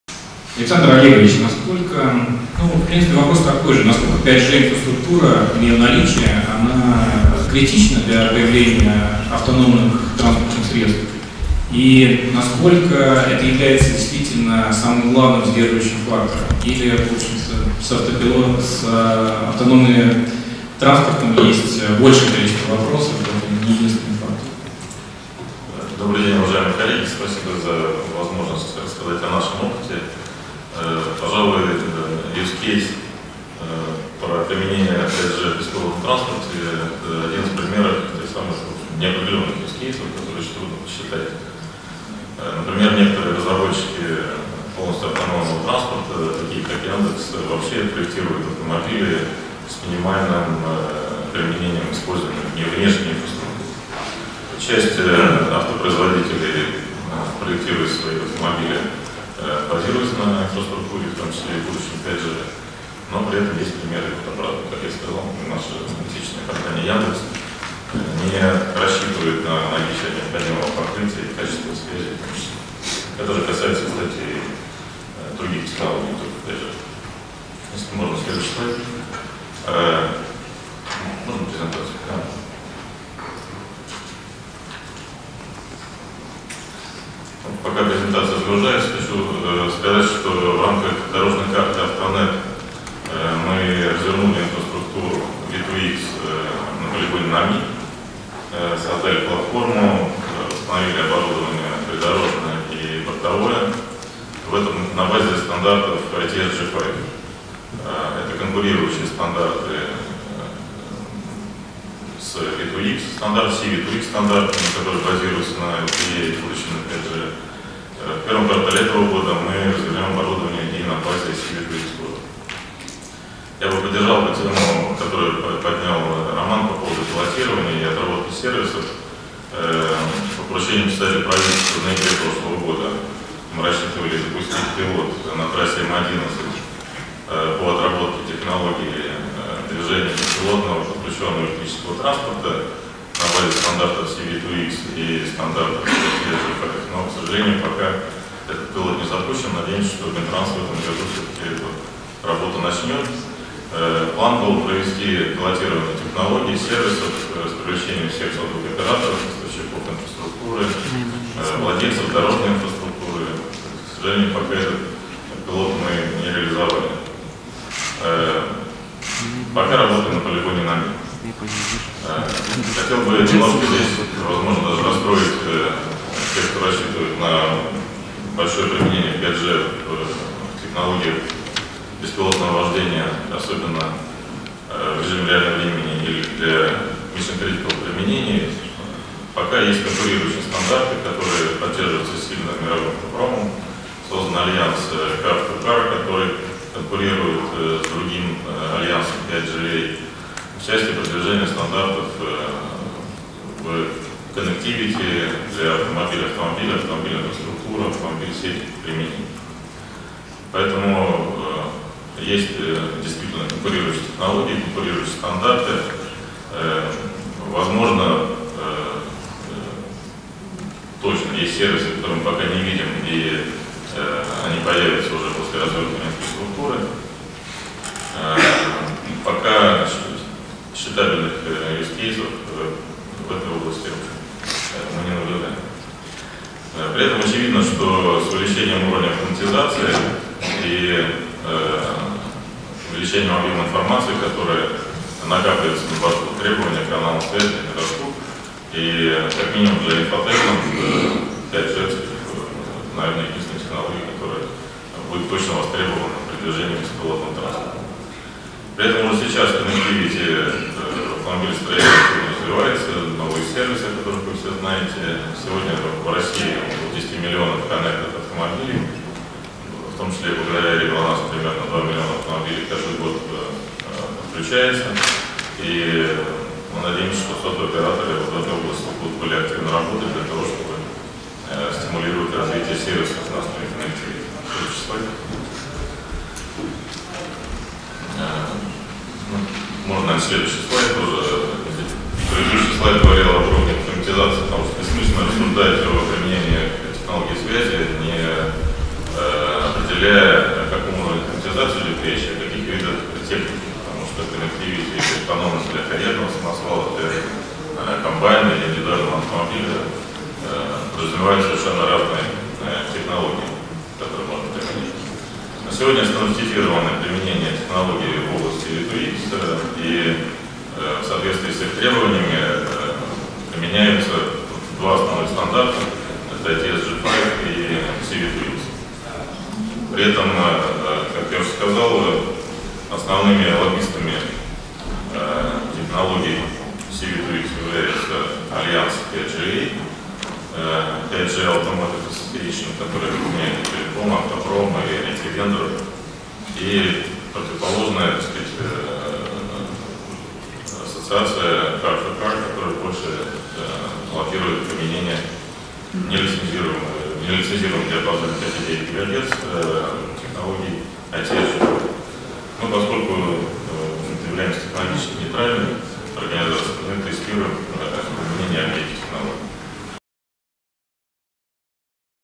5G и транспорт - Гурко, "Глонасс"
Президент НП «ГЛОНАСС» Александр Гурко.
Круглый стол «О перспективах создания и развития сетей связи 5G в Российской Федерации»
30 января 2019. Аналитический центр при Правительстве Российской Федерации.